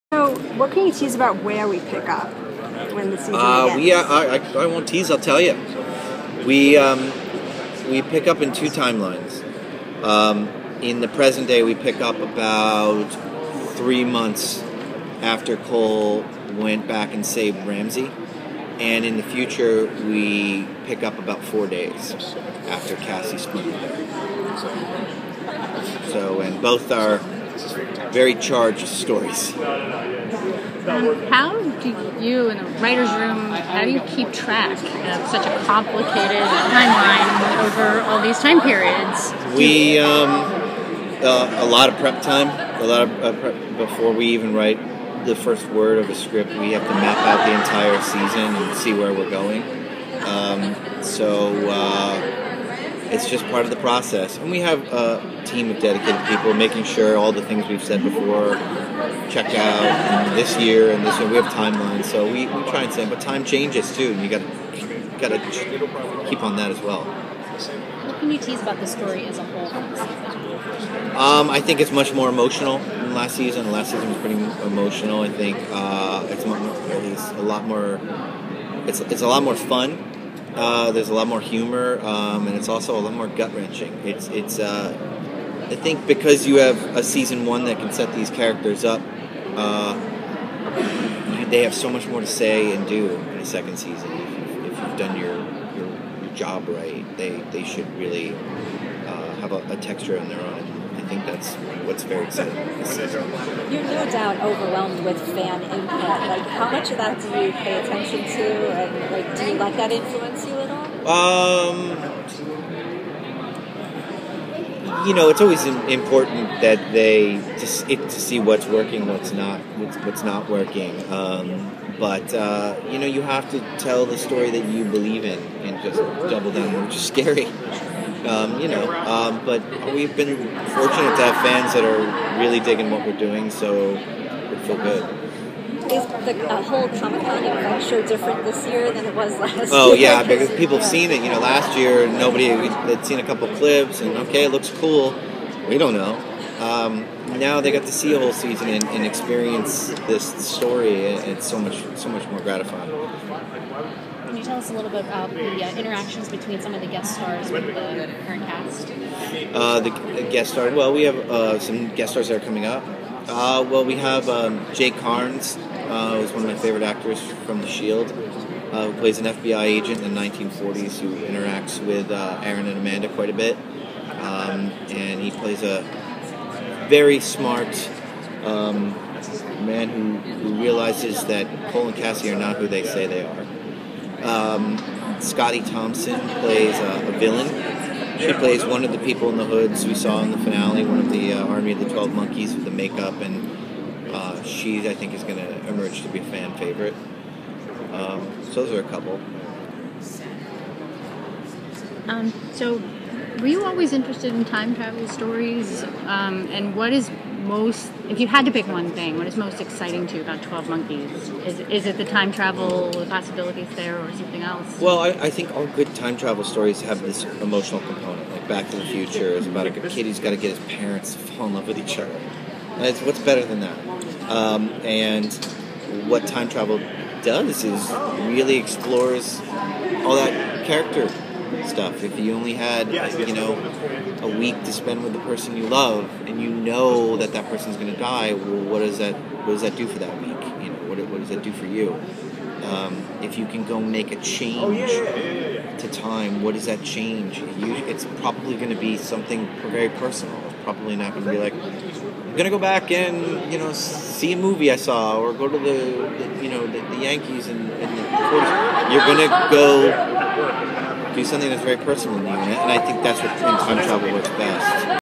Terry Matalas Previews 12 MONKEYS Season 2
Terry Masalas in the 12 Monkeys press room at New York Comic-Con 2015.